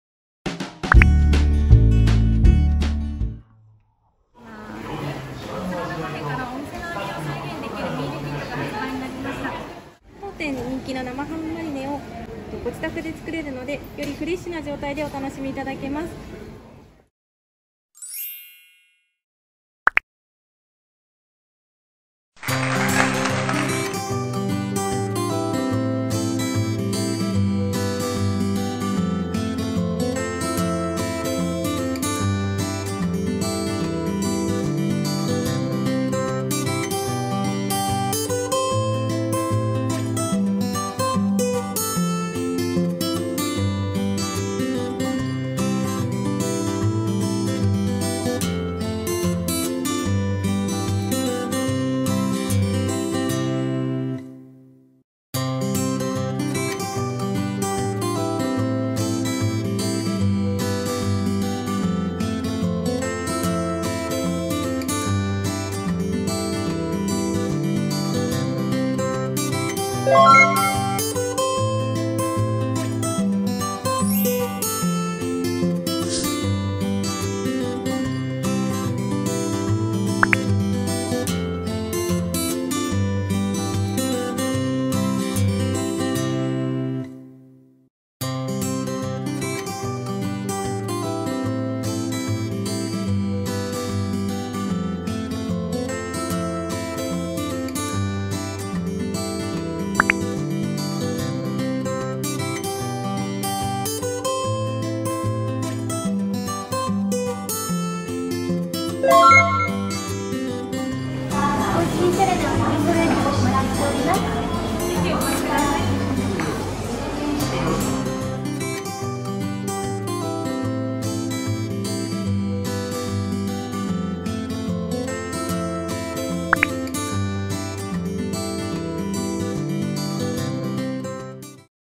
🥗Salad Cafe(サラダカフェ)の【Saladキット】作ってみた🥗生ハムと玉ねぎの柑橘ソースマリネキット編
あなたの街のサラダショップ
Salad Cafe(サラダカフェ)から
サラダのプロの味をご自宅で再現できる
【Saladキット】が発売！！

Salad Cafeの店長が実際に作ってみました⭐️
お家で作るサラダを美しく見せる
盛り付けテクニックは必見です👀！